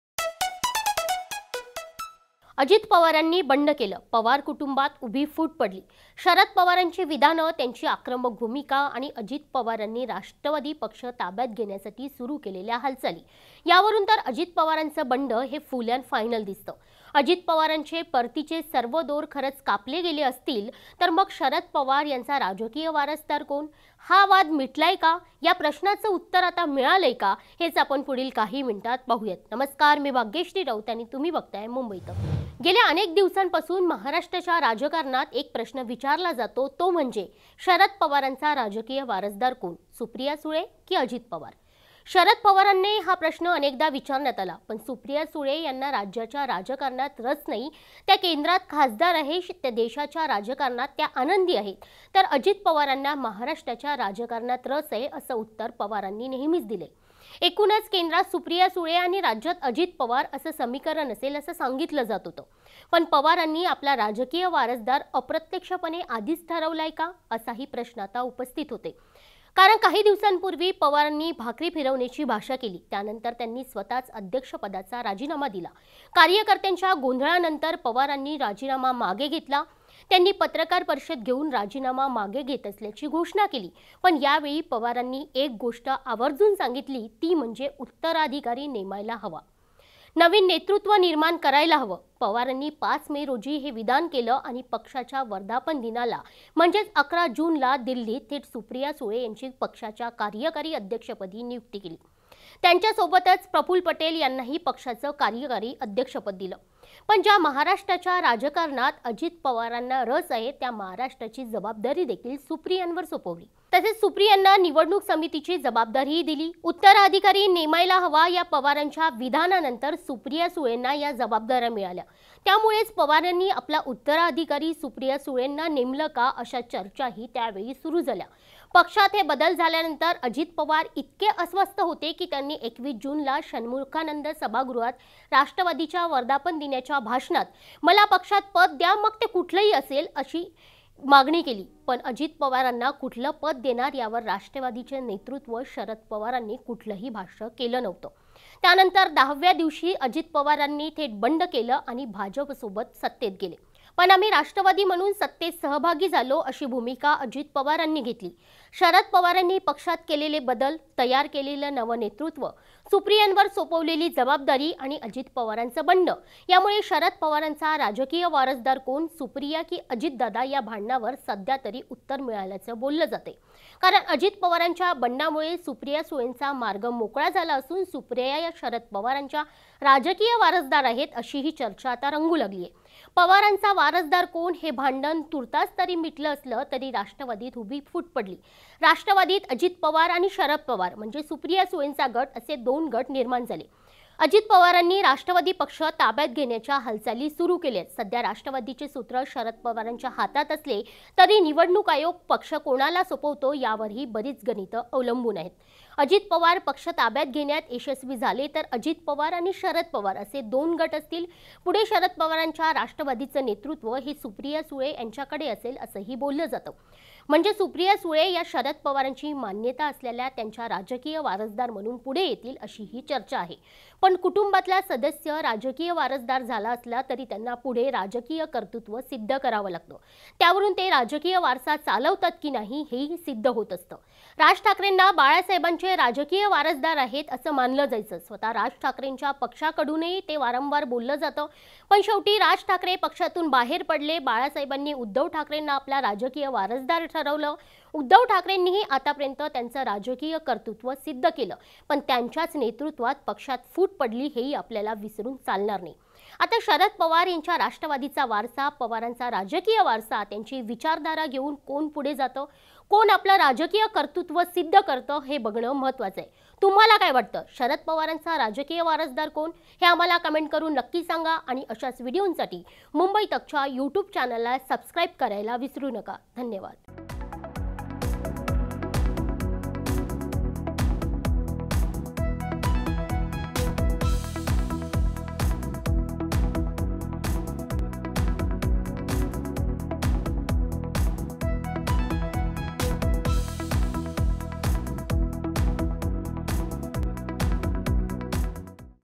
Sharad Pawar यांच्या राजकीय वारसदाराचं भांडण मिटलं का? | Ajit Pawar | Supriya Sule
अजित पवारांनी बंड केलं...पवार कुटुंबात उभी फूट पडली. शरद पवारांची विधानं, त्यांची भूमिका आणि अजित पवारांनी राष्ट्रवादी पक्ष ताब्यात घेण्यासाठी सुरू केलेल्या हालचाली यावरून तर अजित पवारांचं हे फूल अँड फायनल बंड दिसतं...अजित पवारांचे परतीचे सर्व दोर खरंच कापले गेले असतील तर मग शरद पवार यांचा वारसदार कोण? हा वाद मिटला का? या वादावर उत्तर मिळालंय का? हेच पुढील काही मिनिटात पाहुयात...